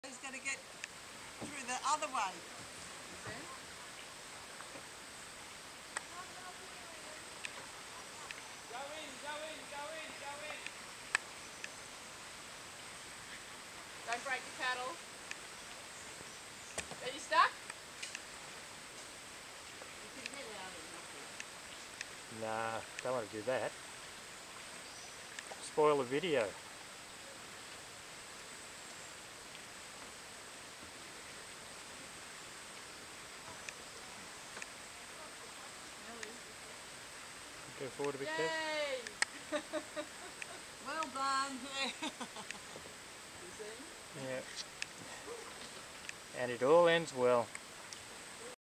Ian paddling at the Bomaderry Creek limit of navigation - March 2017